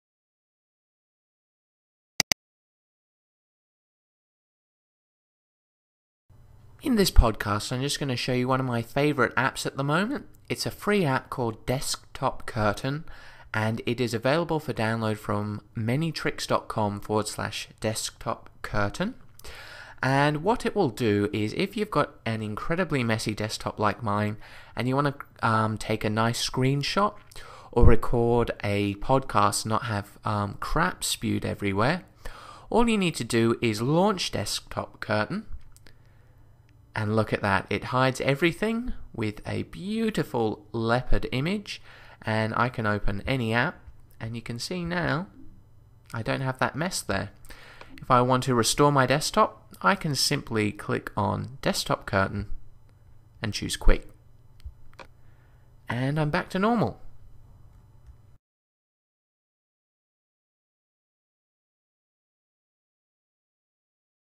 Cool Apps - Desktop Curtain
Need to tidy up your desktop so it looks good in podcasts and screenshots??? This is the answer.